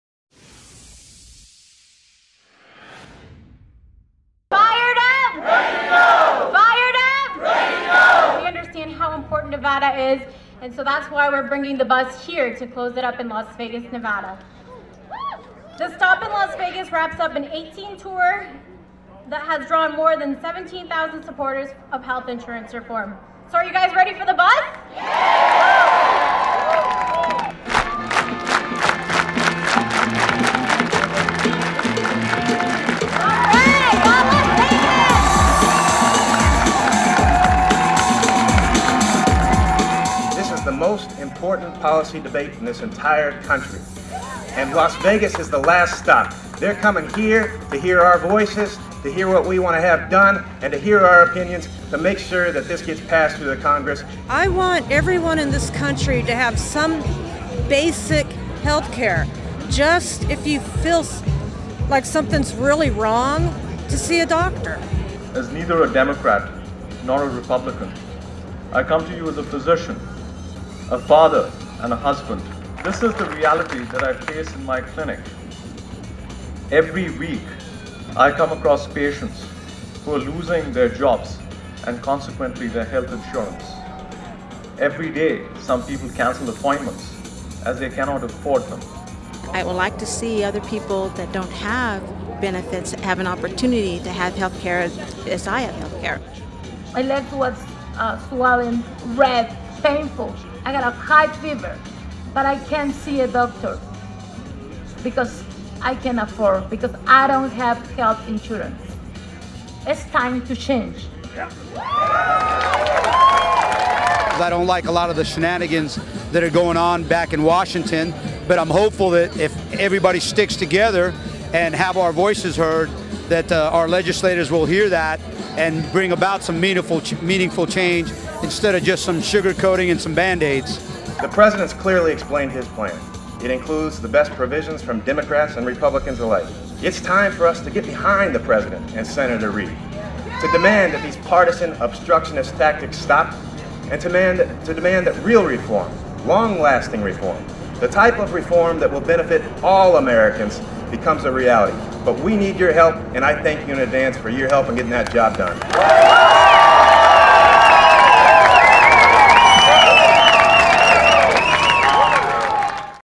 Roadtrip to Healthcare Reform
President Obama's "Health Insurance Reform Now: Let's Get It Done" Bus made it's last stop in Las Vegas Thursday night after 18 months of touring the country to gather support for the healthcare reform bill being debated in Congress. More than a hundred supporters gathered to cheer on the bus as it rolled in to the Culinary Workers Union. Nevada Secretary of State Ross Miller called for bi-partisan support of the bill, while a doctor and local mother told how their lives have been affected by a lack of healthcare reform.